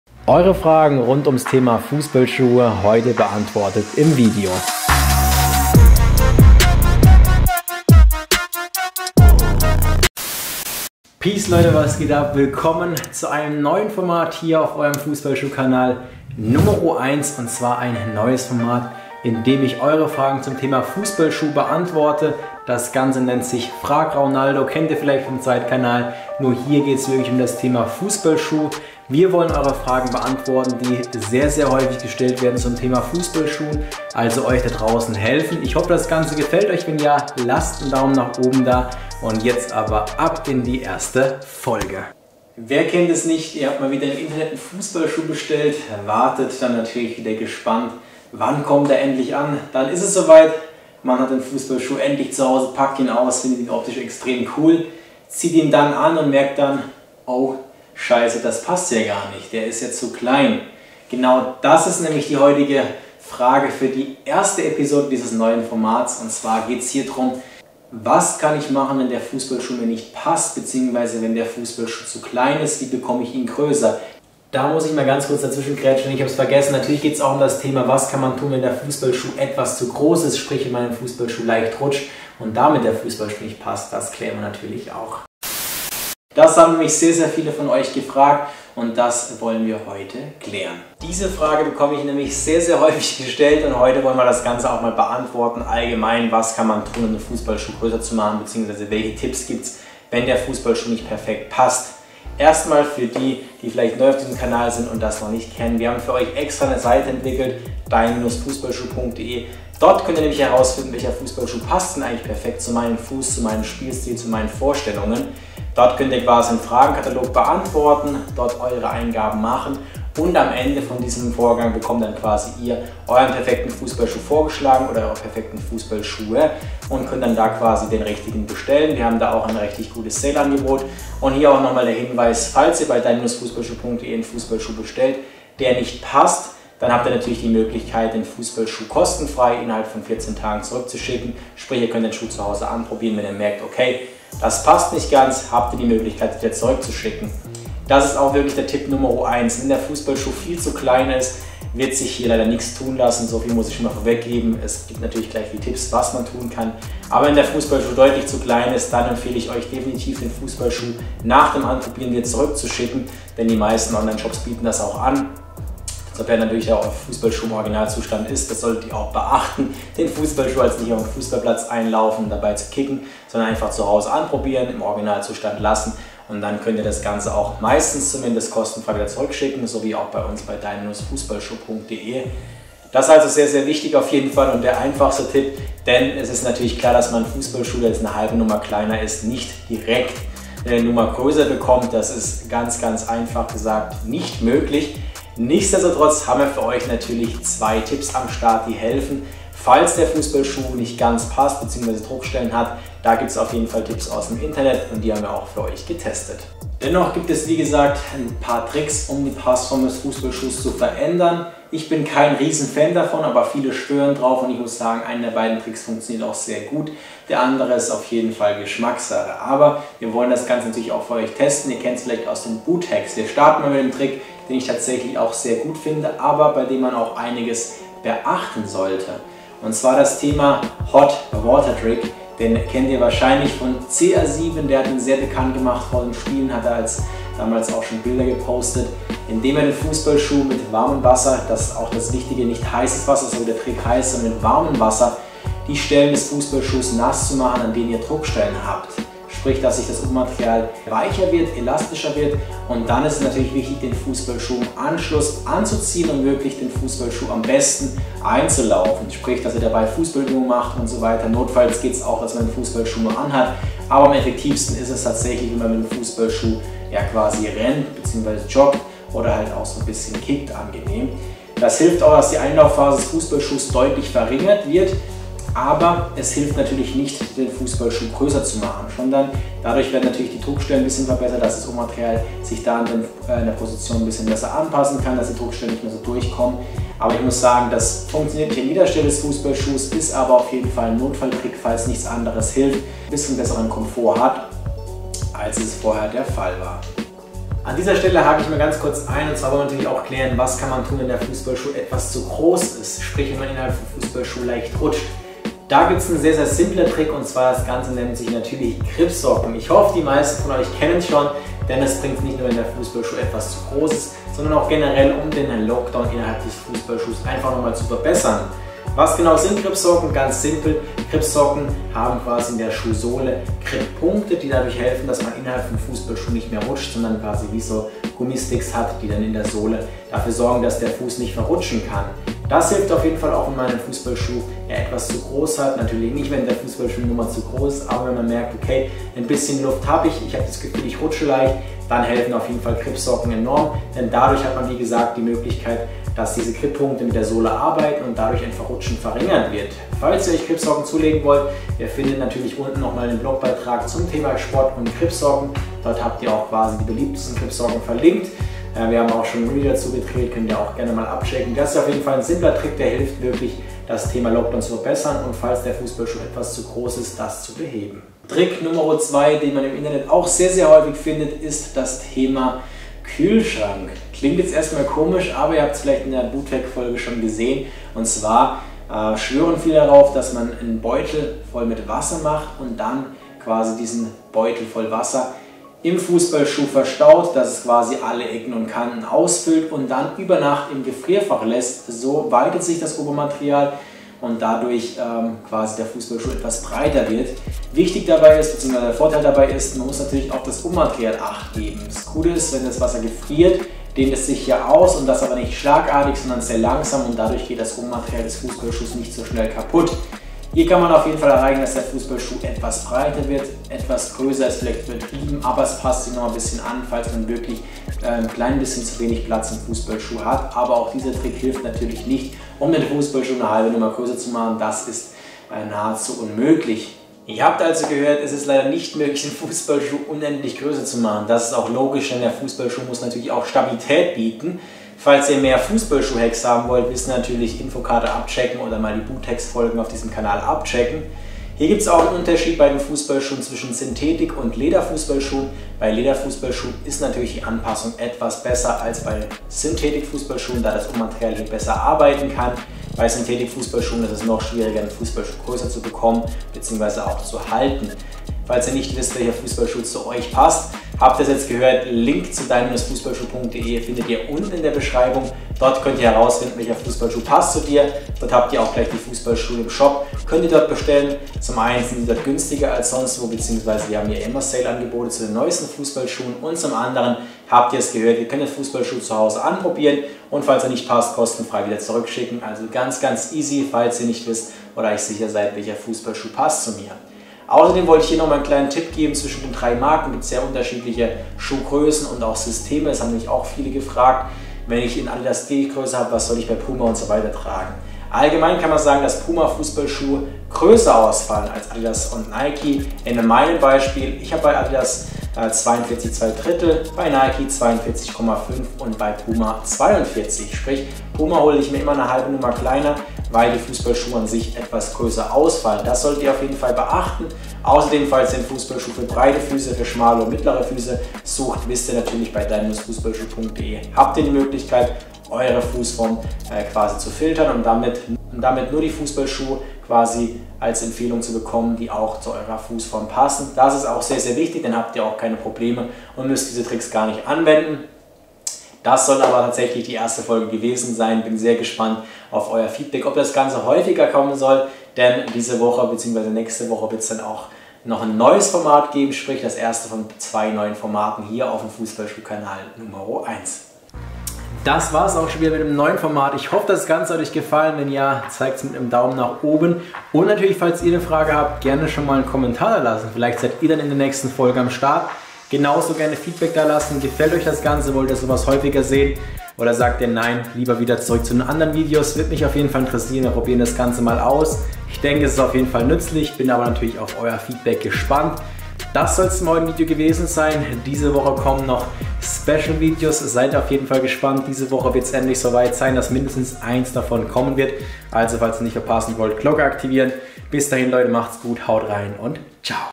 Was tun wenn der Fußballschuh NICHT passt?!
Was kann man tun wenn der Fußballschuh nicht passt?! Egal ob der Fußballschuh zu groß oder zu klein ist, man ärgert sich sehr darüber... Im heutigen Fußballschuh Video wollen wir klären, was man unternehmen kann, wenn der Fußballschuh zu klein oder auch zu groß ist. Wie kann man den Fußballschuh größer machen? Wie kann man das Rutschen im Schuh verhindern, wenn er zu groß ist? Genau das klären wir in der ersten Folge des neuen Formats "Frag Raunaldo", bei dem ich eure Fragen zum Thema Fußballschuhen beantworte!

*Werbung
Finde heraus welcher Fußballschuh zu dir passt: